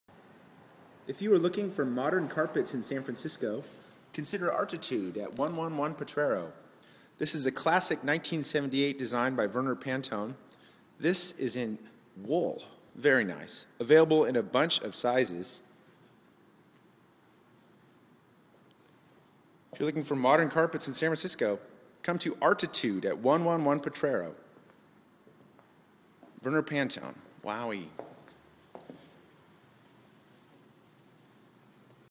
Modern Carpet San Francisco
If you're looking for modern carpets in San Francisco, please visit Arttitud at 111 potrero for classic designs in luxurious materials like silk or wool.  Design pictured is by Vernor Panton, 1978.